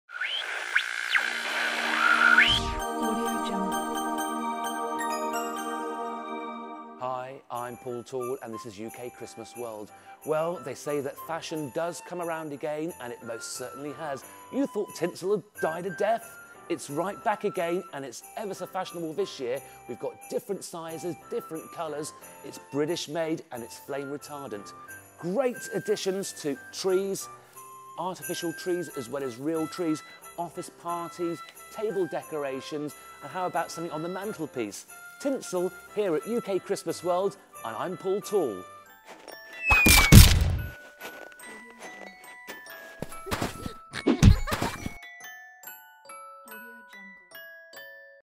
Premium Quality Tinsel - UK Christmas World
Paul Toole, the King of Christmas Lights shows off our new range of premium quality tinsel, all made here in the UK!

A one stop shop for all your Christmas needs, from lights to decorations and trees. UK Christmas World